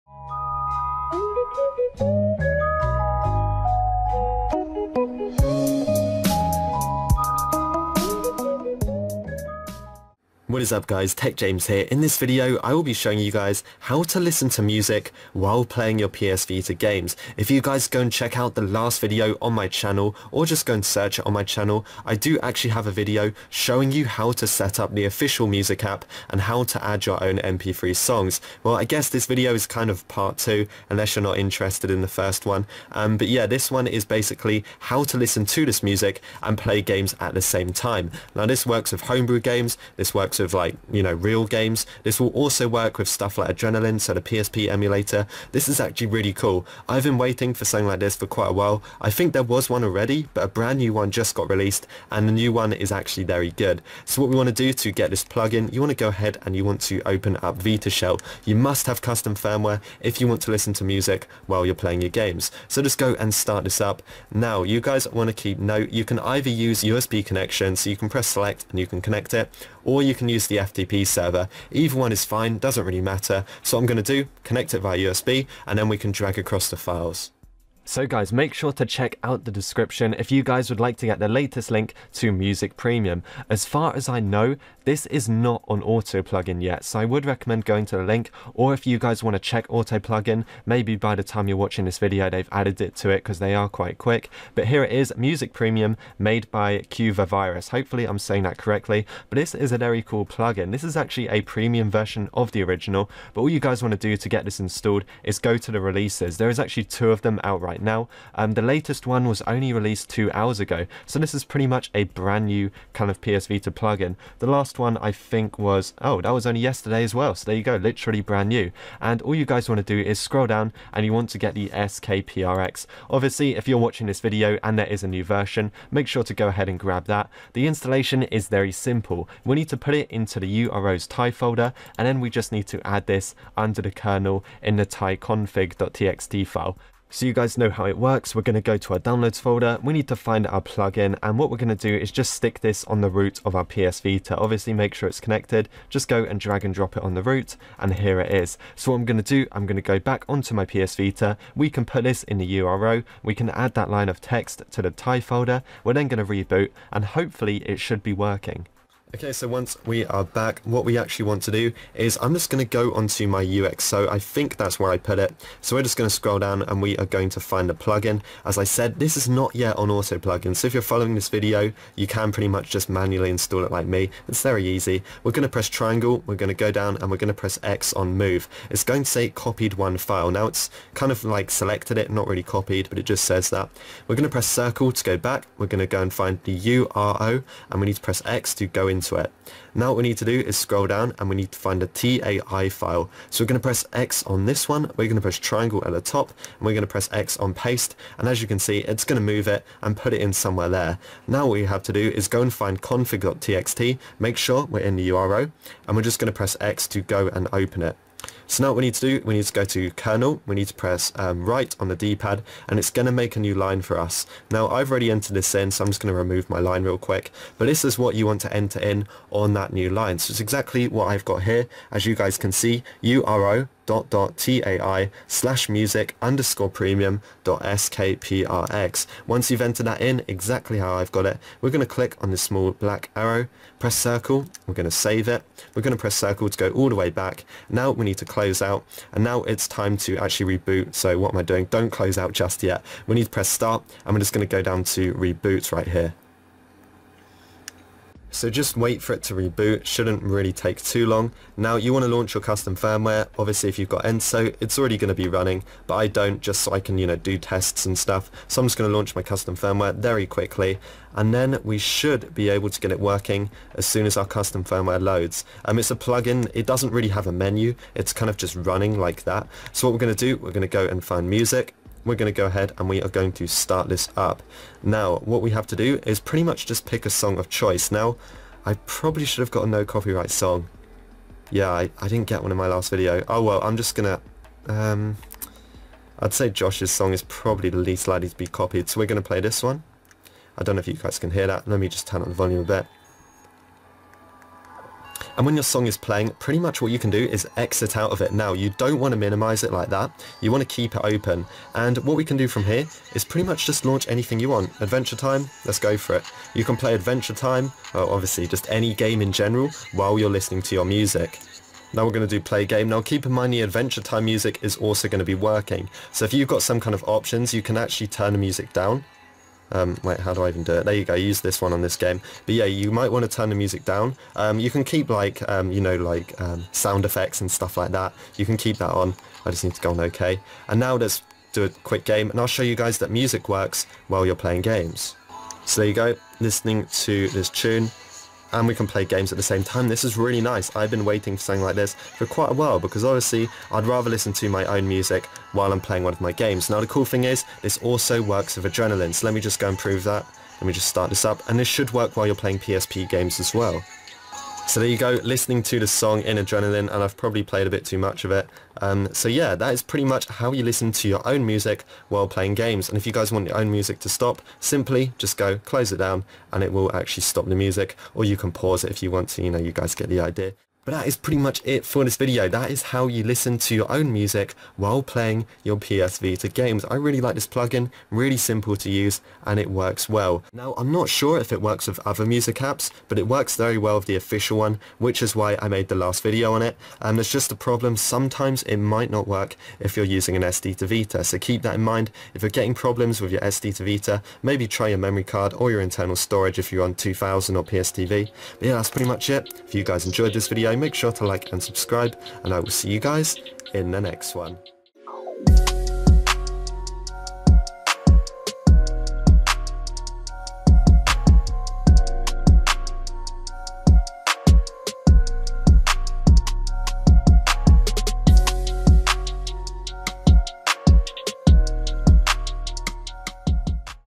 PS Vita Play Music While Playing Games/Adrenaline/Emulators! (Music Premium)
How to play your PS Vita music while playing any game including the adrenaline PSP emulator! This also fixes the error “The Audio Has Been Paused” This plugin is called the Music Premium Plugin!

➤ (8GB Memory Card)

➤ (Official Vita Power Bank)

This video can be removed.